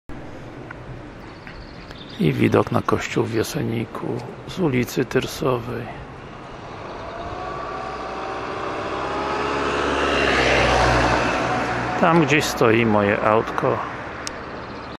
Jesenik Tyrsova 1
Widok na Kościół w Jeseniku z ulicy Tyrsova

Pensjonaty w Jeseniku

Jesenik Tyrsova  Dom wypoczynkowy w Jeseniku. Niedaleko jest ulica Lipovska, niedaleko rzeczki. Zwraca uwagę ciekawa architektura. Obiekt po jakimś remoncie. Po drodze mijałem całe mnóstwo takich domów. Wycieczka ciekawa dla miłośników spaceru
Część wypoczynkowa miasta, a wille są bardzo interesujące.